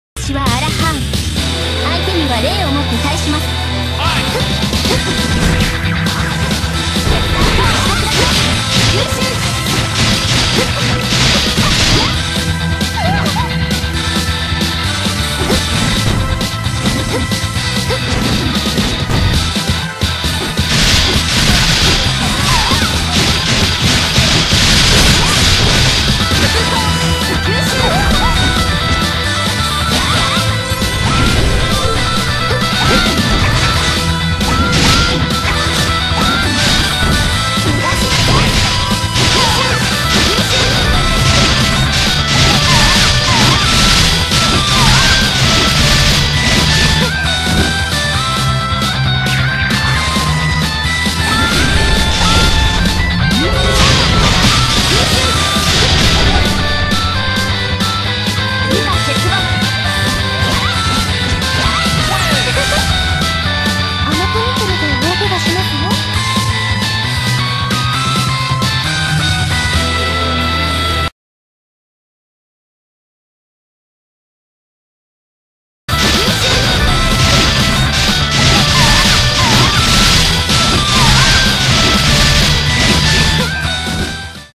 Another Rof user complaining (So nothing new here)
Traducción: Sabes, es curíoso que digas eso por que fuiste tú quien me dio el mp para empezar.
¿Recuerdas esto?, pues ahí tienes el mp que use para joderte.
Además tú usaste RoF y yo no me queje.
Derpy derp